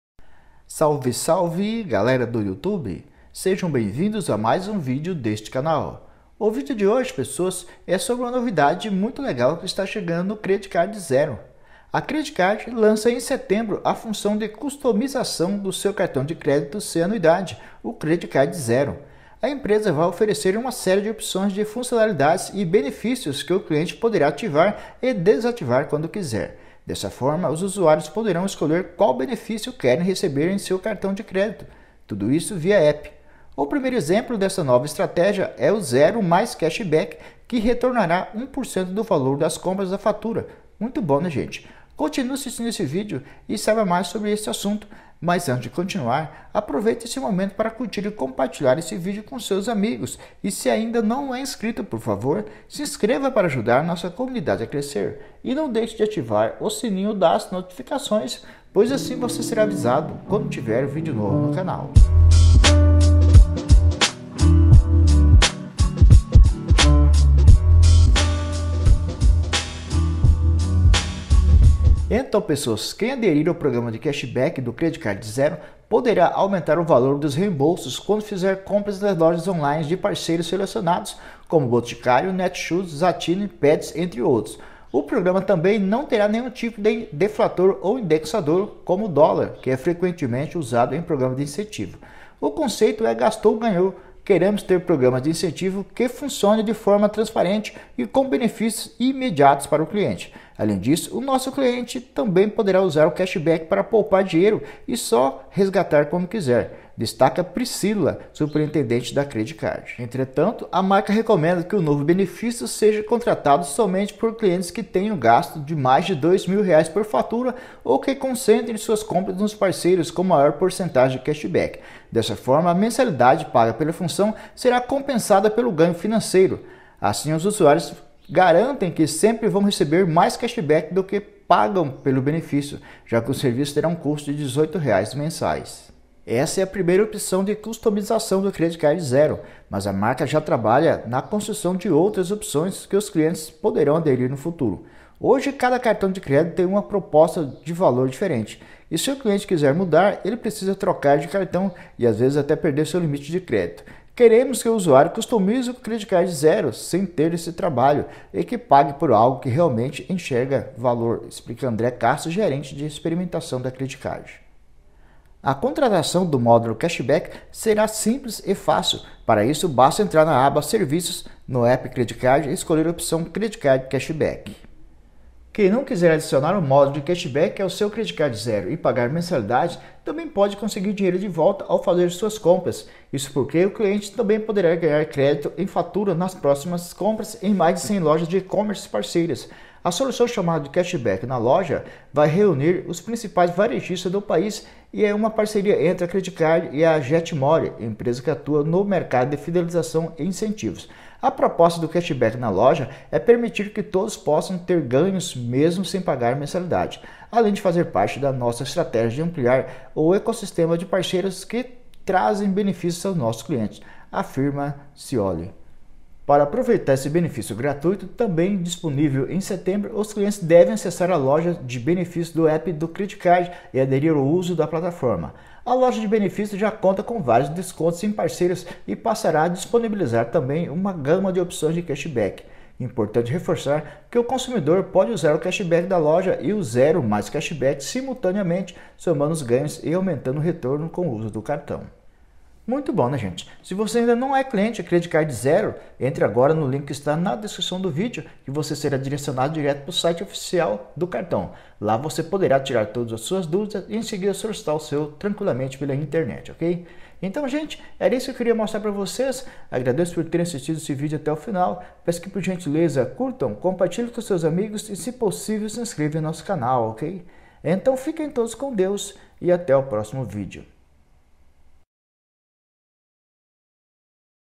CredicardZero sai na frente e lança cashback de 1% para todas as compras feitas no cartão,confira
A Credicard lança em setembro a função de customização de seu cartão de crédito sem anuidade, o Credicard ZERO. A empresa vai oferecer uma série de opções de funcionalidades e benefícios que o cliente poderá ativar e desativar quando quiser.
Para solicitar esre cartão entre no link abaixo;